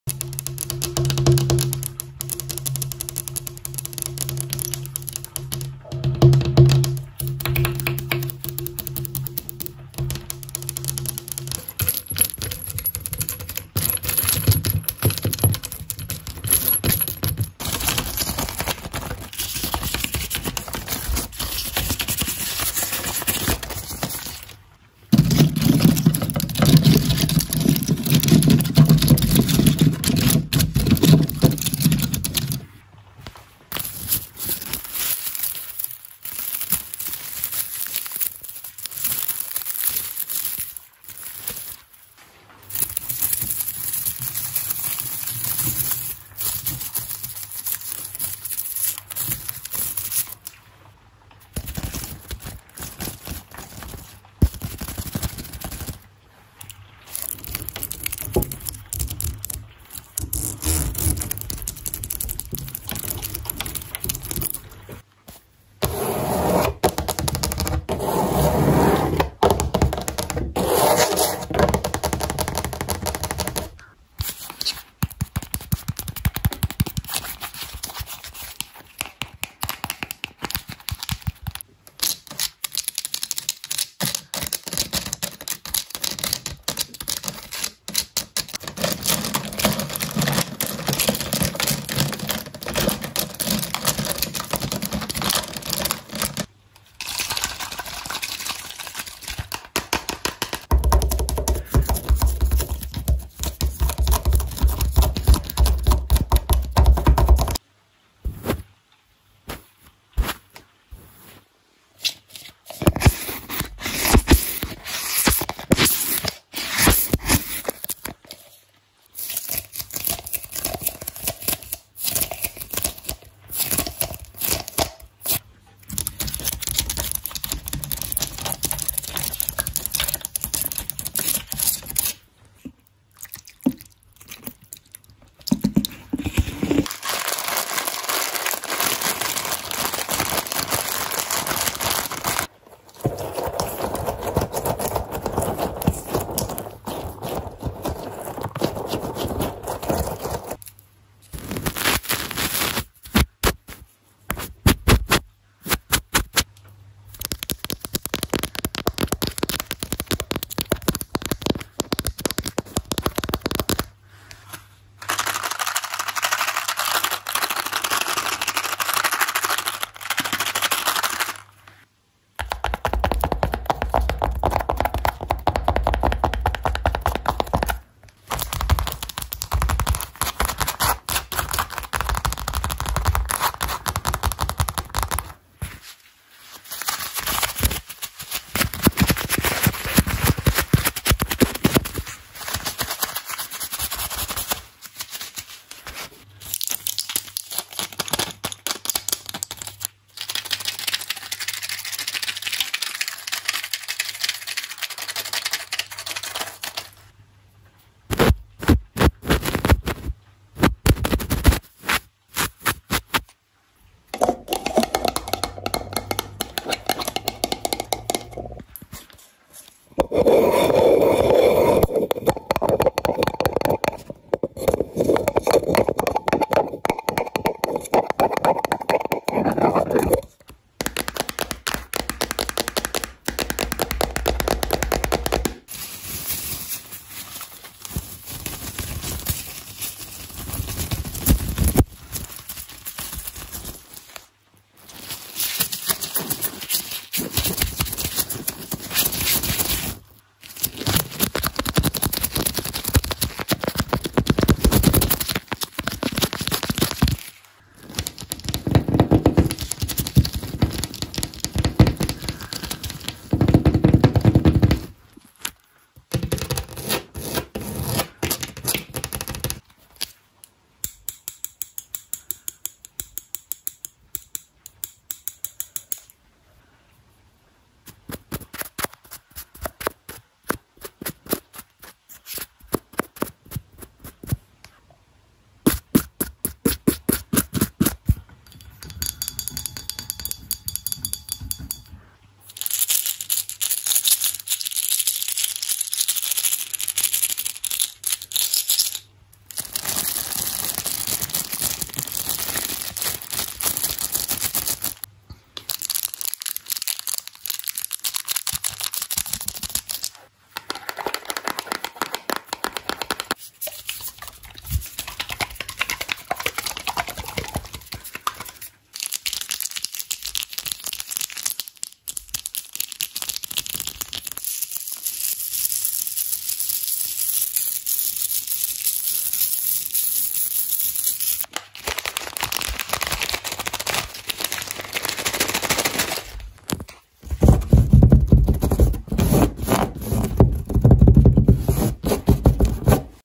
ASMR | 50+ FAST and AGGRESSIVE random triggers!
♥Hello everyone! I really hope you enjoyed this video and you got tingles. Write your opinion in the comment ♥

✨ Inspired by video Eevy ASMR✨